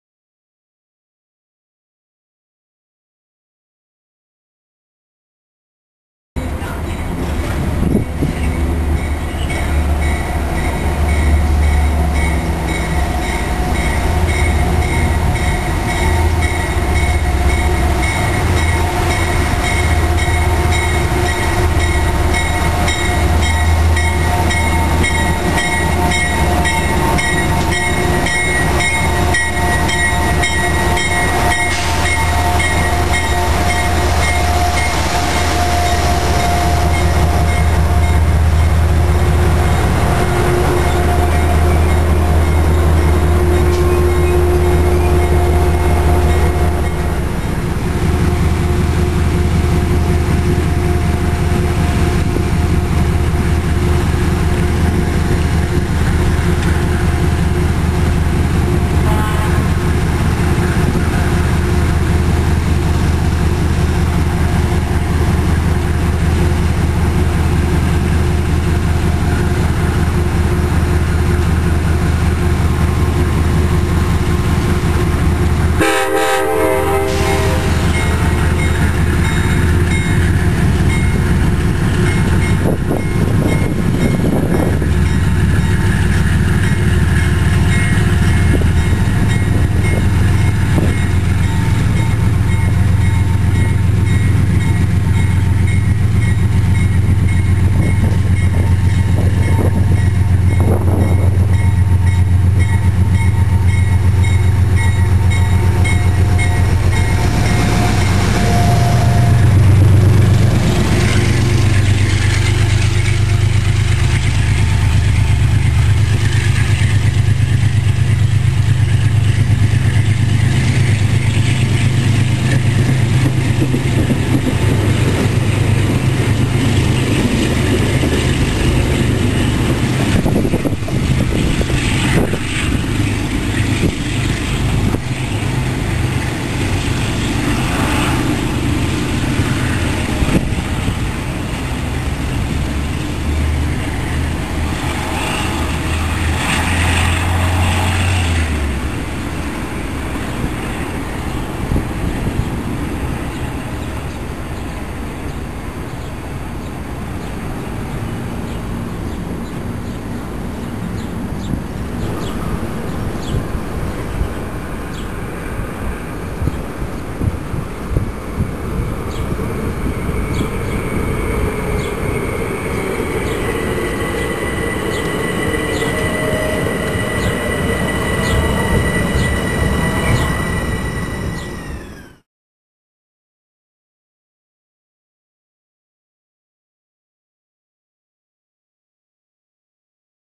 Amtrak Empire Service #283 @ YNY
Amtrak NYP-NFN (New York Penn to Niagara Falls, NY) Empire Service train #283 makes its arrival at Yonkers, NY right on time.

The train is led today by GE P32ACDM #714 as it eases to a stop at Metro North's Yonkers station on the Hudson Line just 15 miles into its journey up the majestic Hudson River before relaying at Albany and turning west to run along the Mohawk River Valley. to points west...